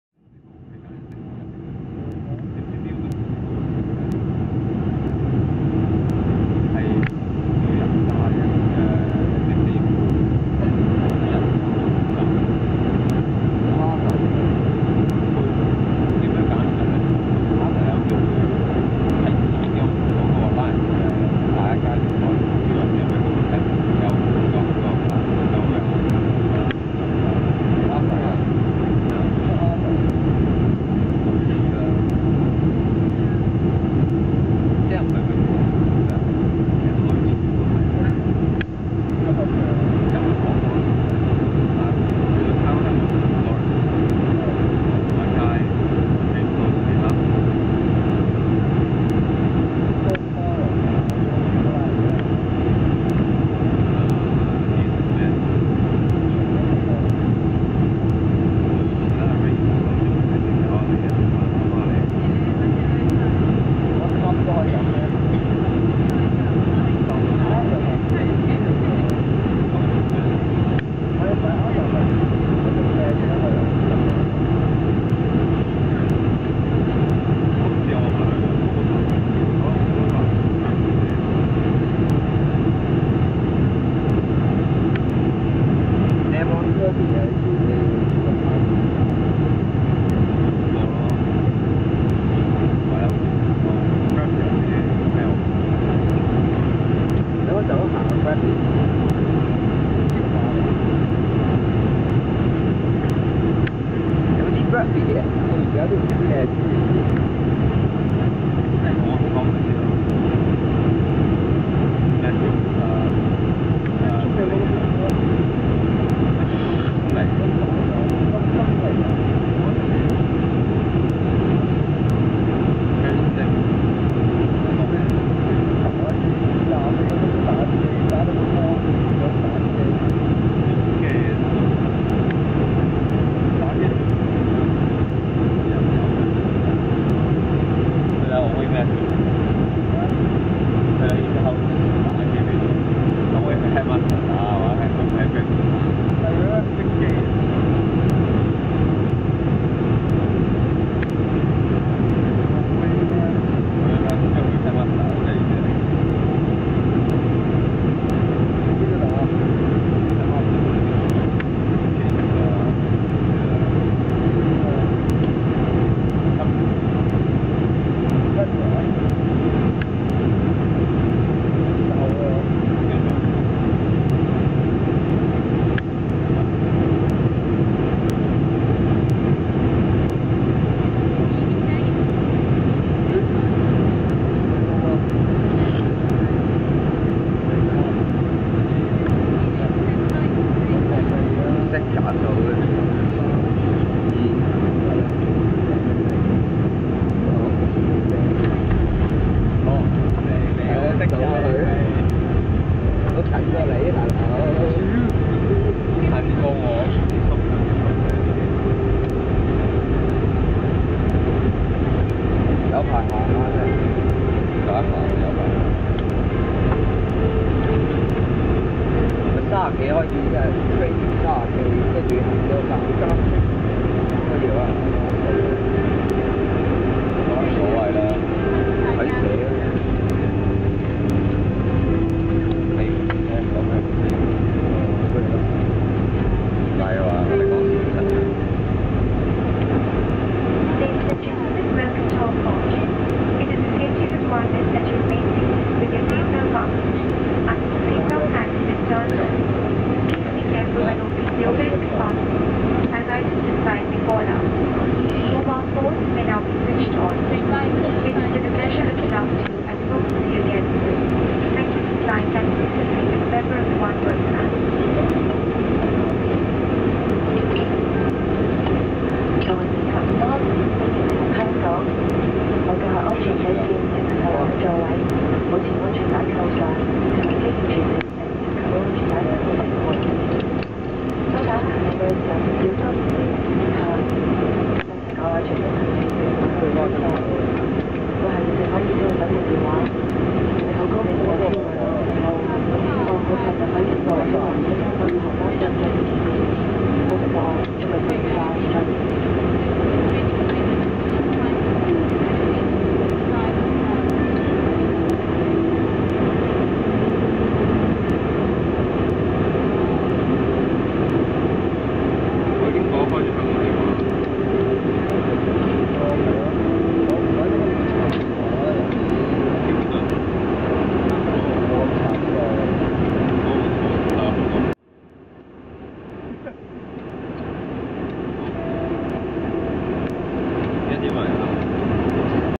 [5] Cathay Pacific Boeing 747 landing Hong Kong International Airport from London in the afternoon
Date & Time: 28th March, 2010 (1:48)
Sit back and enjoy the landing.